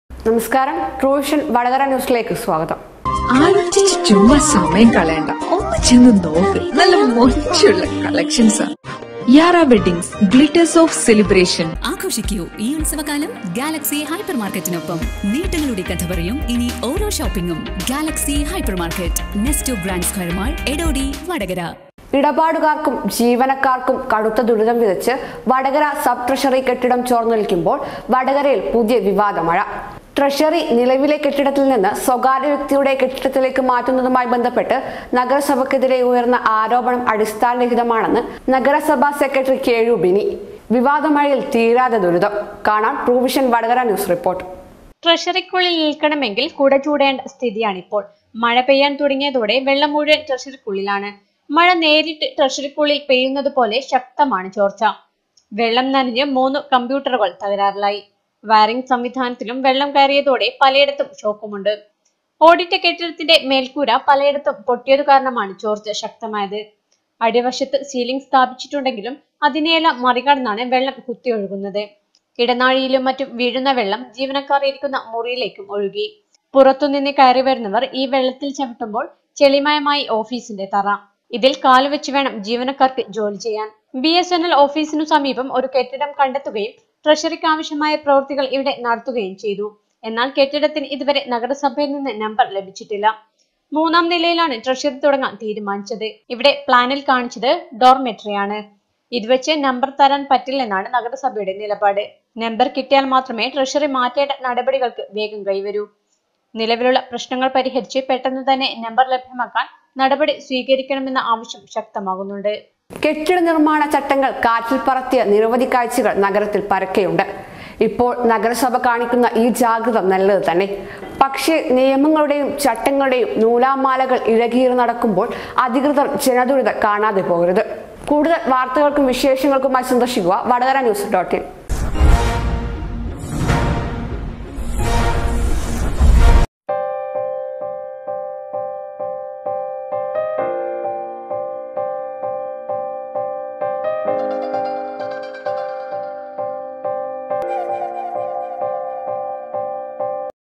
ഇടപാടുകാർക്കും ജീവനക്കാർക്കും കടുത്ത ദുരിതം വിതച്ച് വടകര സബ് ട്രഷറി കെട്ടിടം ......
ഇടപാടുകാർക്കും ജീവനക്കാർക്കും കടുത്ത ദുരിതം വിതച്ച് വടകര സബ് ട്രഷറി കെട്ടിടം ചോർന്നൊലിക്കുമ്പോള്‍ വടകരയില്‍ പുതിയ വിവാദ മഴ. ട്രഷറി നിലവിലെ കെട്ടിടത്തിൽ നിന്ന‌് സ്വകാര്യവ്യക്തിയുടെകെട്ടിടത്തിലേക്ക് മാറ്റുന്നതുമായി ബന്ധപ്പെട്ട് നഗരസഭക്കെതിരെ ഉയർന്ന ആരോപണം അടിസ്ഥാനരഹിതമാണെന്ന് നഗരസഭാ സെക്രട്ടറി കെ യു ബിനി. വിവാദ മഴയില്‍ തീരാ ദുരിതം ... കാണാം ട്രൂവിഷന്‍ വടകര ന്യൂസ്‌ റിപ്പോര്‍ട്ട് .